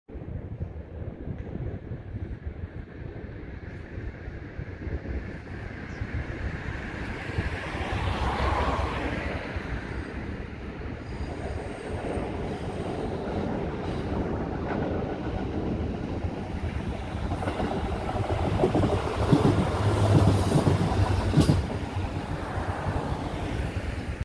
2021/11/8 普通列車(青色キハ220+赤色キハ220)走行@日田～光岡間(花月川橋梁)
2021/11/8　JR久大本線日田～光岡間の花月川橋梁を走行する久留米行き普通列車(シーサイドライナー色キハ220+赤色キハ220_2両)を撮影しました。